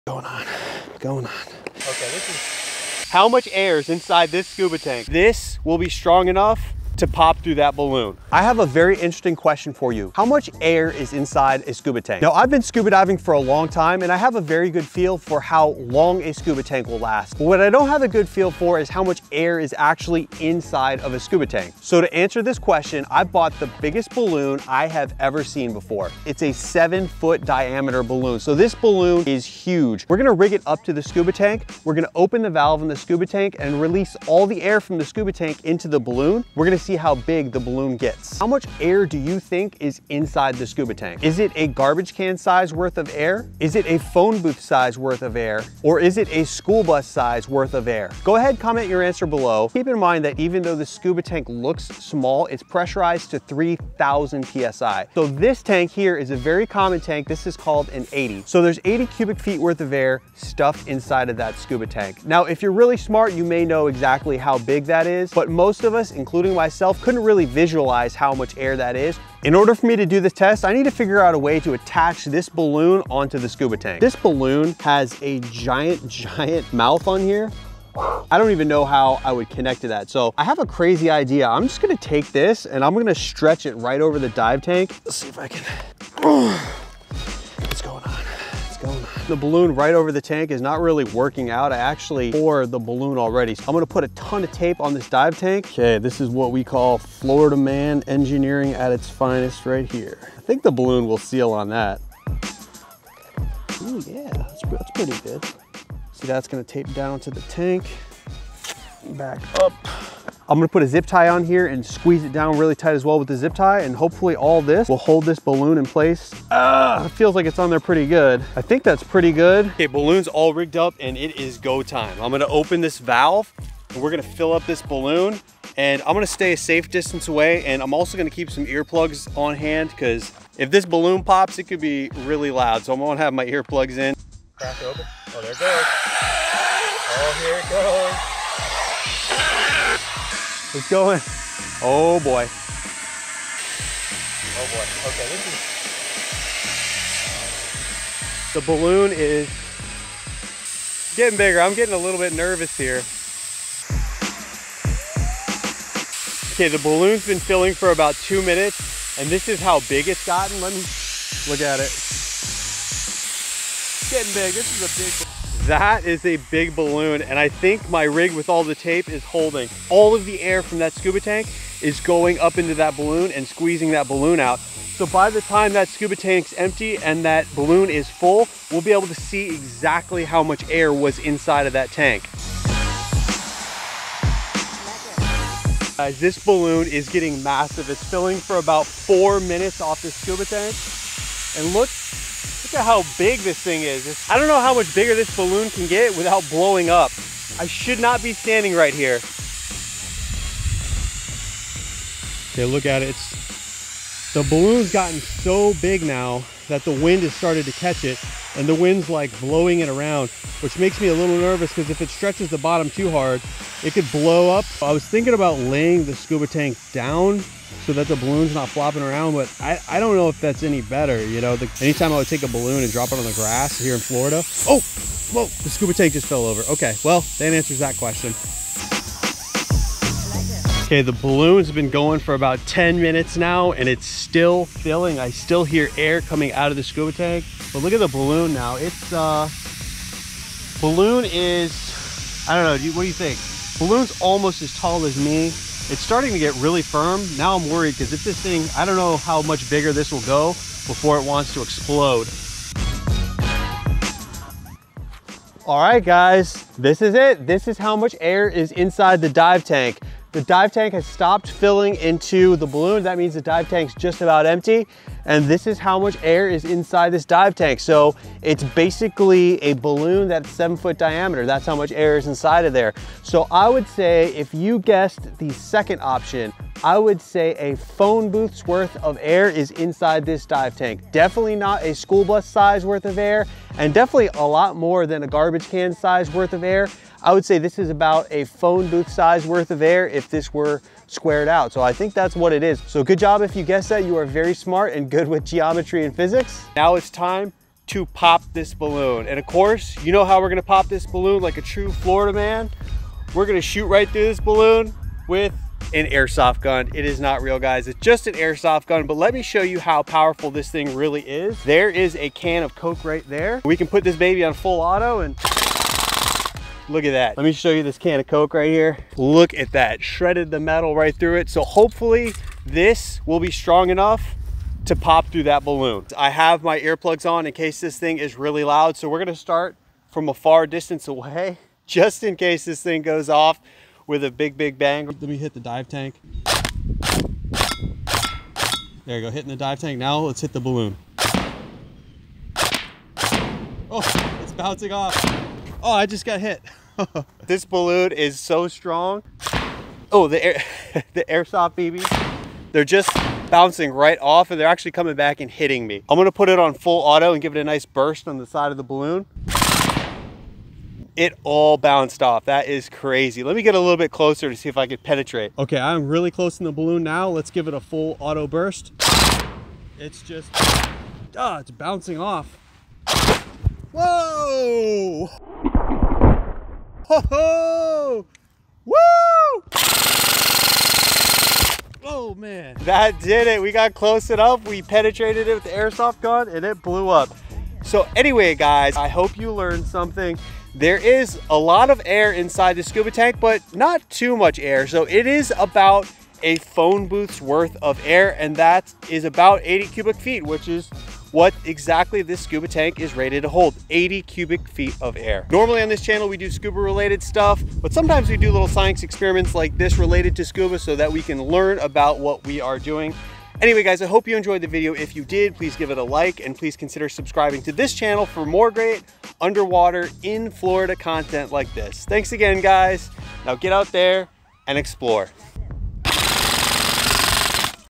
How Much Air is Actually in A SCUBA Tank?
I found out how much air is really in a scuba tank! I go scuba diving alot and have breathed air from many scuba tanks, I have always wondered how much air was actually inside the scuba tank though. I mean, if you let all the air out of the scuba tank how much space would it fill or the Total Uncompressed Volume. For this scuba science experiment I got a giant balloon and attached it to the scuba diving tank and released all the air into the balloon. The balloon inflated to appx. 5.5 feet in diameter, using the formula to find the volume of a sphere, I roughly calculated the amount of air that was in the balloon. It was around 80 cu. ft. which is exactly what this particular dive tank is rated to hold. I hope you enjoy the vid and learning how much air is actually in a Scuba tank.